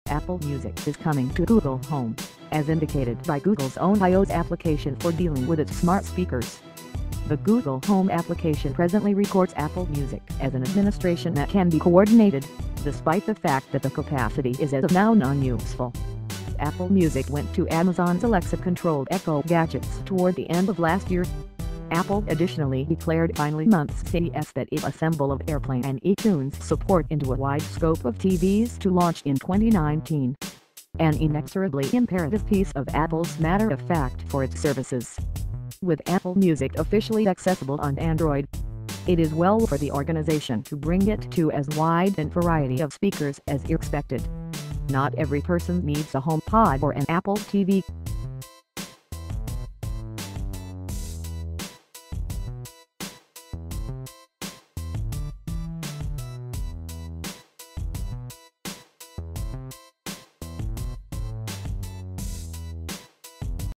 Today’s tech news: Google Home now supports Apple Music
Apple Music is coming to Google Home, as indicated by Google’s own iOS application for dealing with its smart speakers.